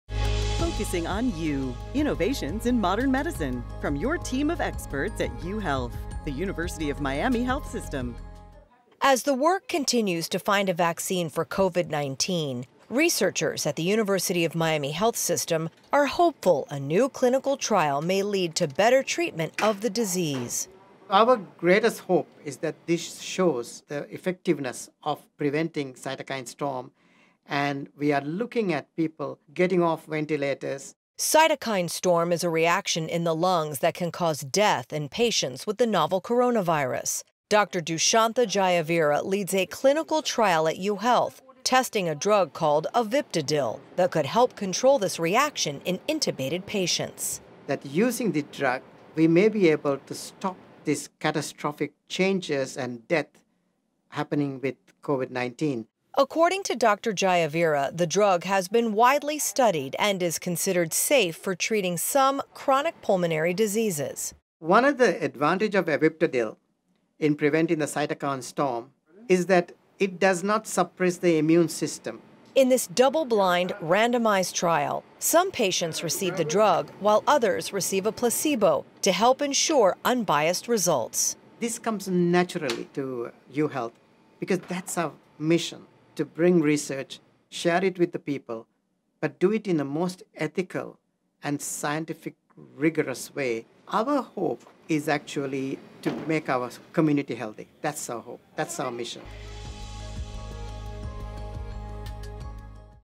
Focusing on You: UM Tests Drug to Lessen Severity of COVID-19
Dr. Dushyantha Jayaweera, physician-researcher at University of Miami Health System, discusses a clinical trial for a drug called Aviptadil that may help prevent death and serious illness due to COVID-19.